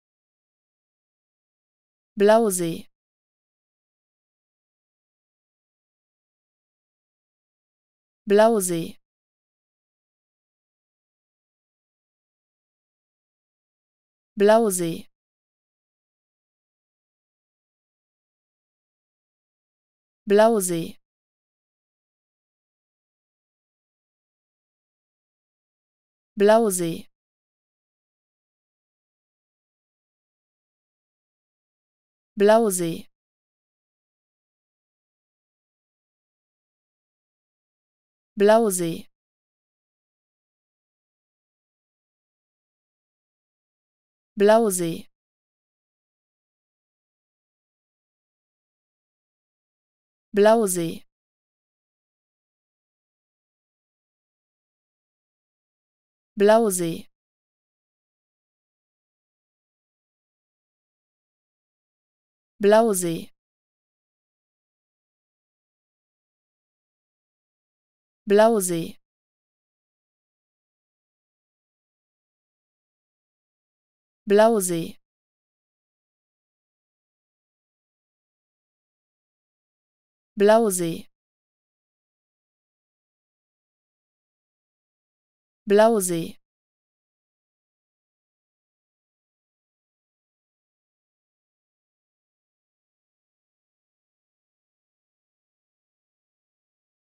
How to Pronounce ''Blausee'' Correctly in German
Learn how to say ''Blausee'' in German with this short pronunciation tutorial.